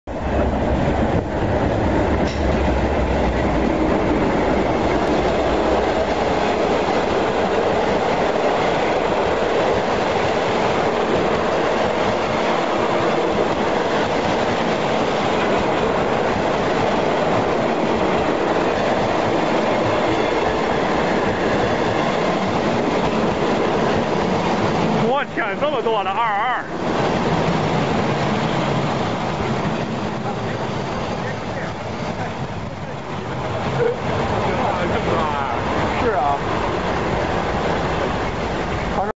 2009.03.16 南京-南昌K505超级混编大埋汰进南京南（现中华门）站停车。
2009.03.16 南京-南昌K505超级混编大埋汰进南京南（现中华门）站停车。